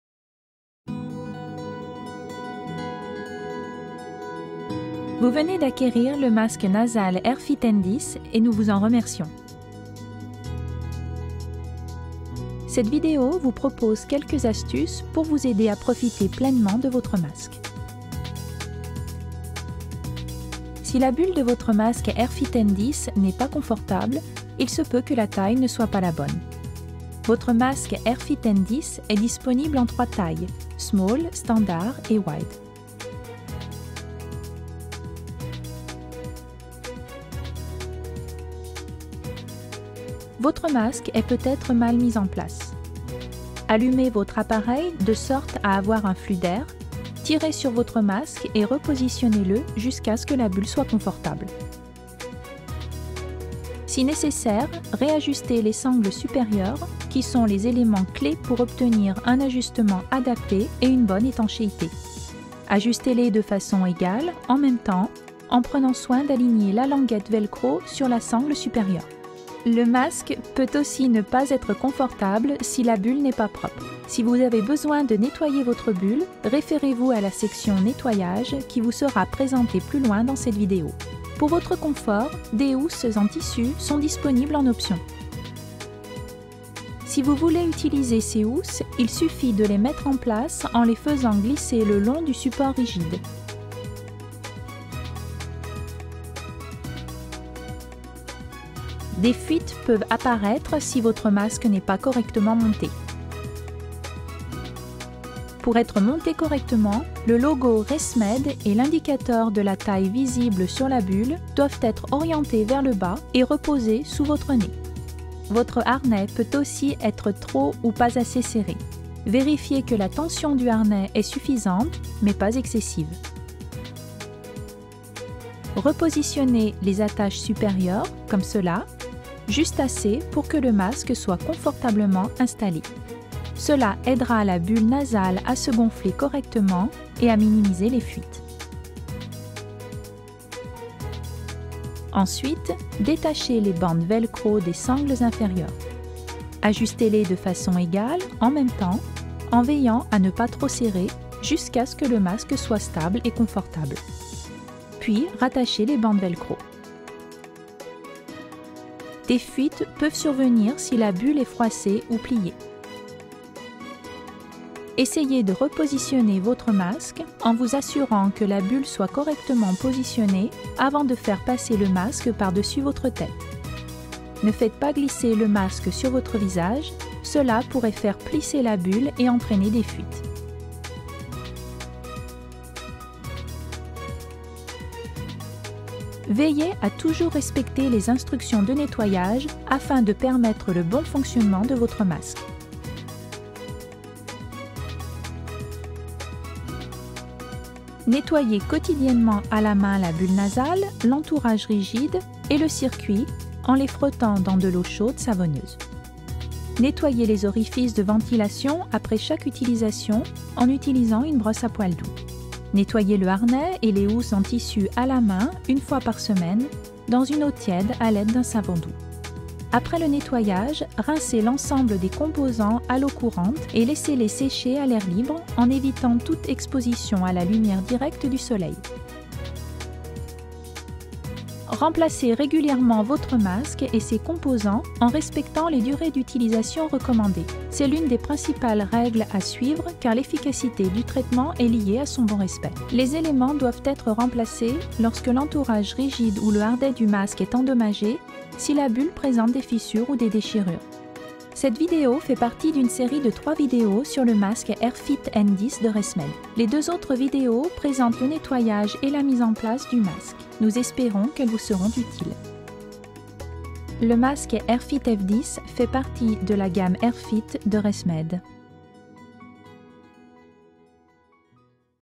Nos réponses à vos questions sur le masque nasal AirFit™ N10
ResMed répond à vos questions concernant votre masque nasal AirFit N10. Voici quelques conseils pour ajuster correctement votre masque. Vous y trouverez des instructions de nettoyage et de montage. Apprenez à quel moment remplacer votre masque.

pour en savoir davantage sur ce masque nasal.
Le masque nasal AirFit N10 est le résultat de 25 ans d'innovation dans le domaine des masques. Sans support frontal, il vous offre une liberté visuelle pour toujours plus de confort. Ses clips magnétiques Easyclick  permettent de mettre le masque de façon simple et rapide.

ResMed Air Solutions. Un nouveau départ pour un sommeil profond.